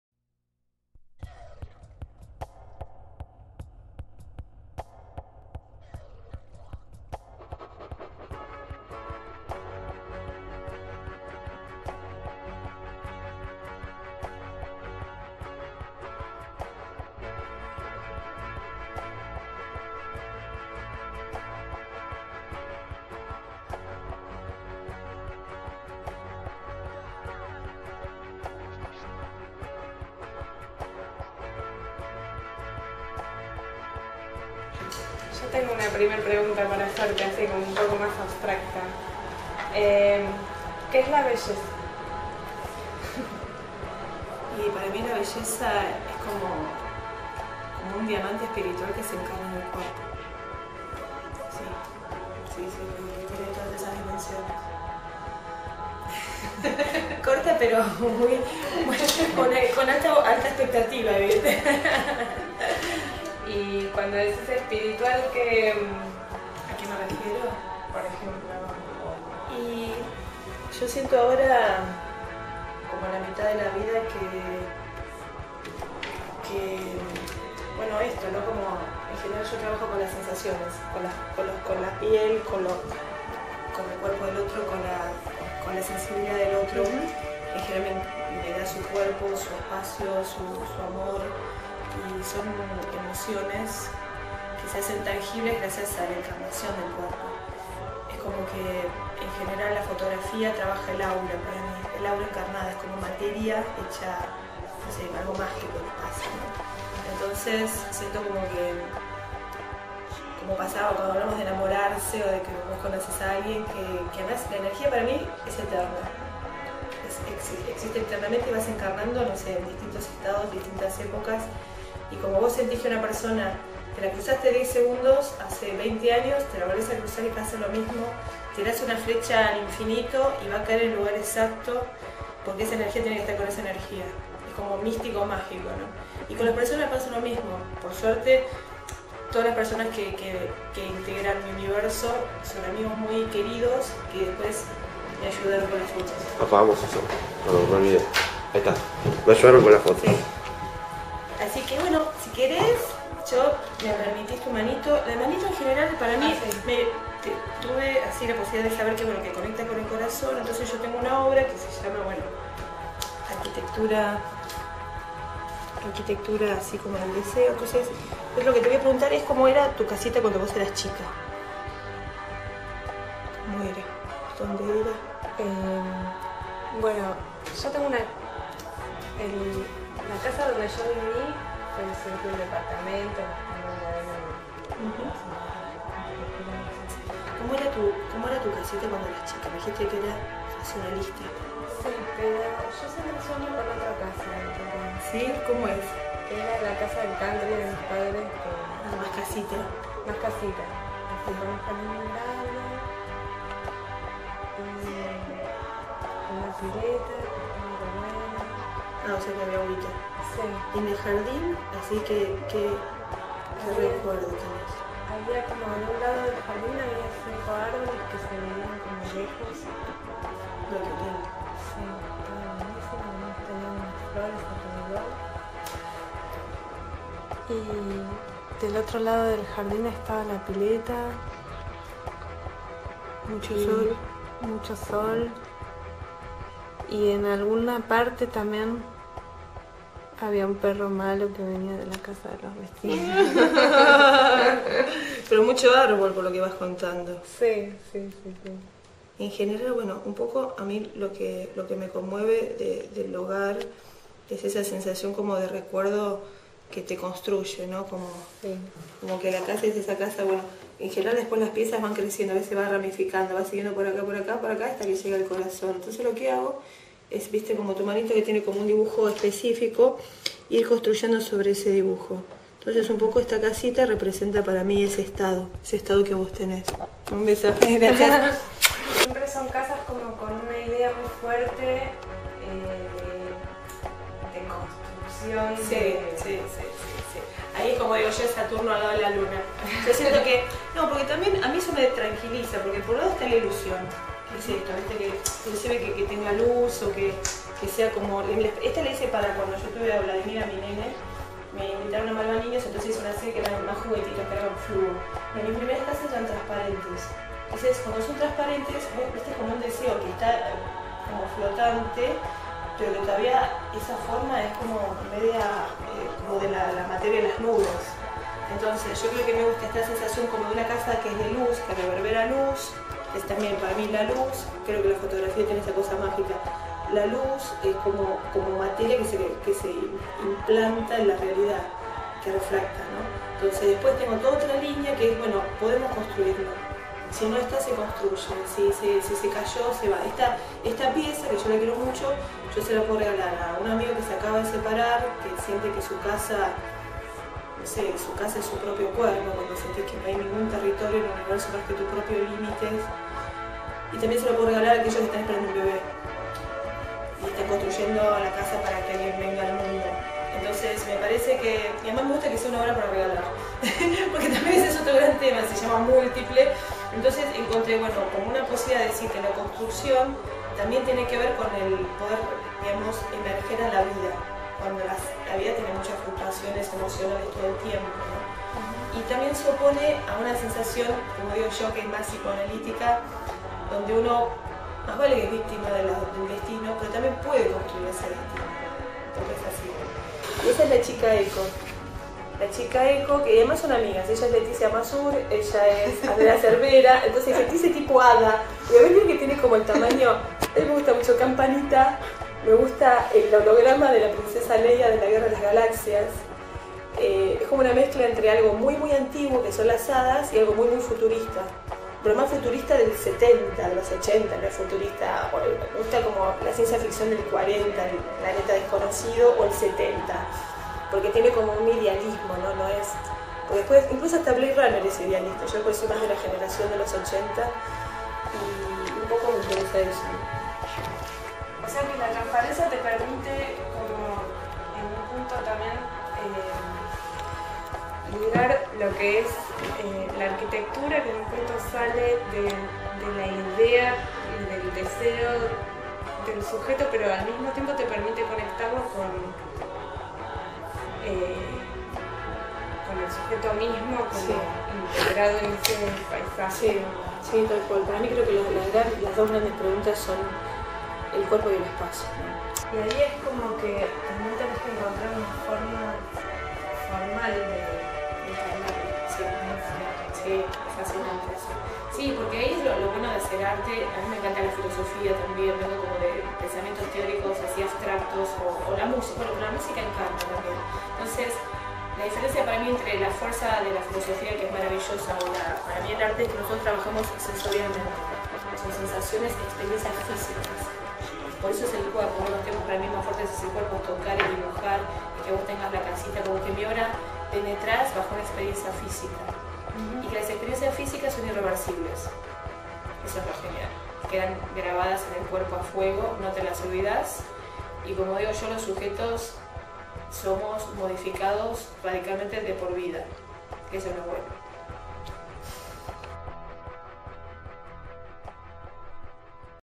Juanele interviews Fabiana Barreda
Juanele visited artist, curator and professor Fabiana Barreda in her studio. She tells us her ideas about beauty and how bodies displayed in space become the uncertain territory of fluctuating emotions.